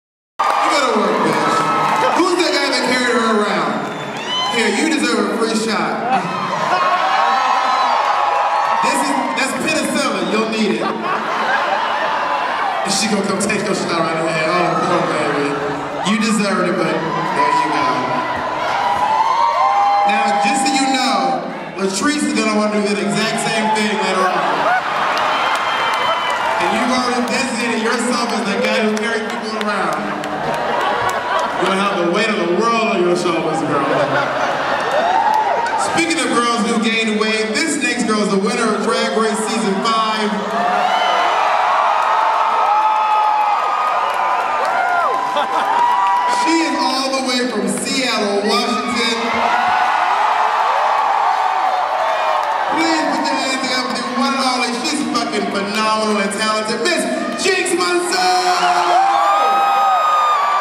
All Stars Live - Bob the Drag Queen/ Detox Banter
Detox & Bob the Drag Queen at The Belasco theatre for "All Stars Live" on Friday, April 28th, 2017!